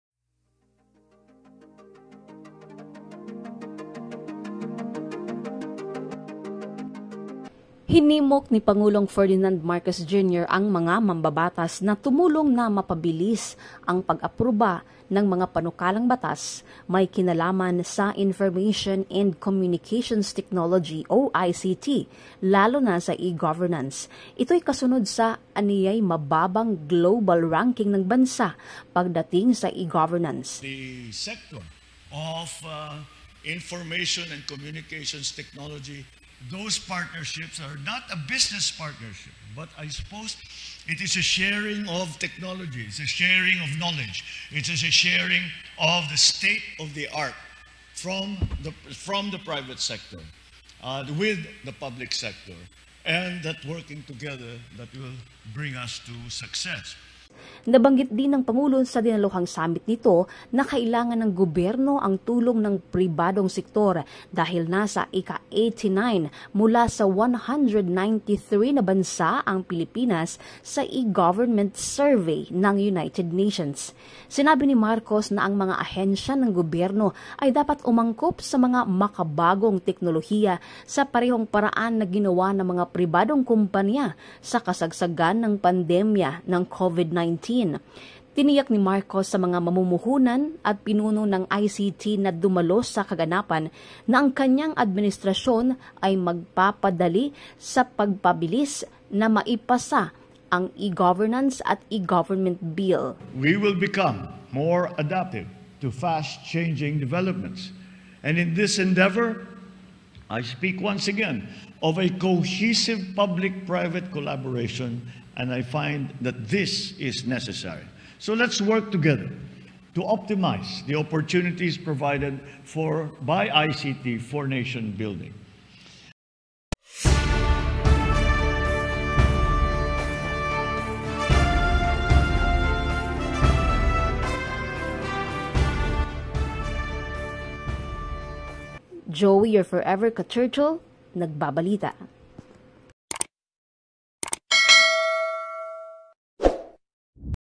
President Marcos tututukang mapabilis ang pagpasa ng mga ICT bill ng bansa.
E-governance at E-government bill ng bansa, prayuridad na maipasa ng administrayon, ayun kay President Bongbong Marcos

Video & Pictures :RTVM
                   & World Class-CPIO10101